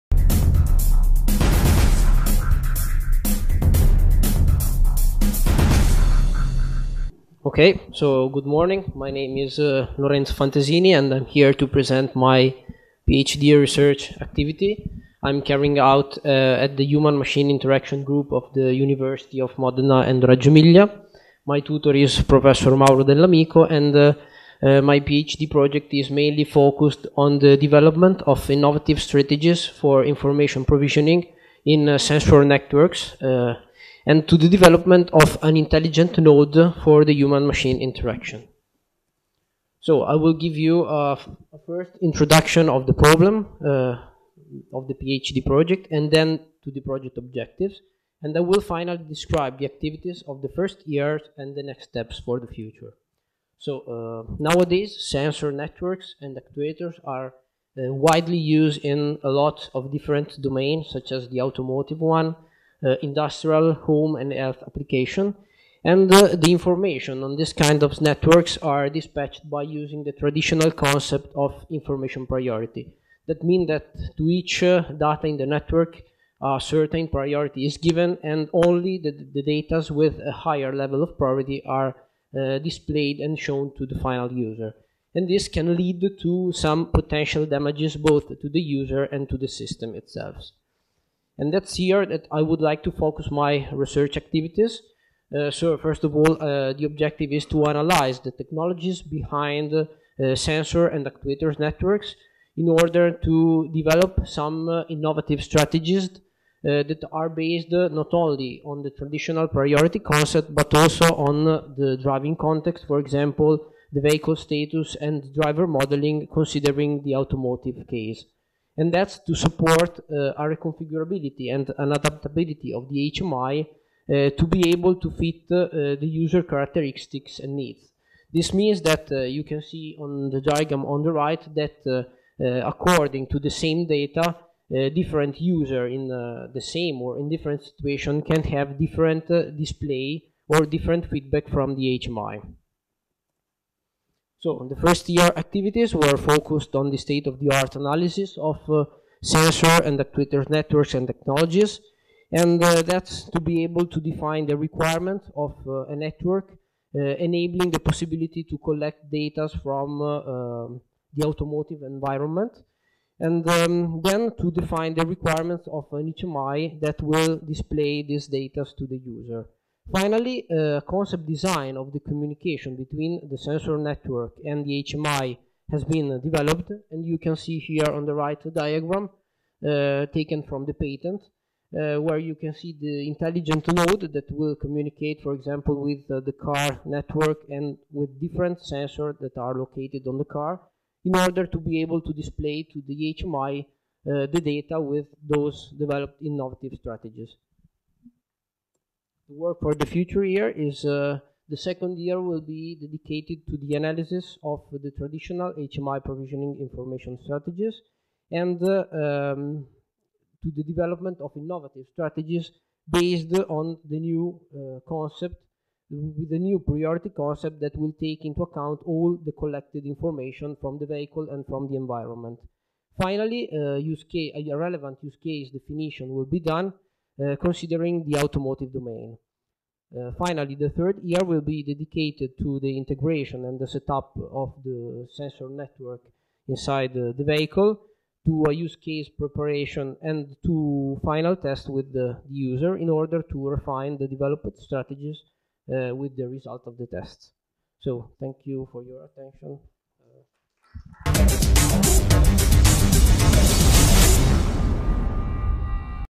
37) Lorenzo Fantesini
D-Day 2011
Development of Innovative Strategies for Information Provisioning in Sensors Networks
21-22 febbraio 2011, ore 9.00
Modena, Facoltà di Ingegneria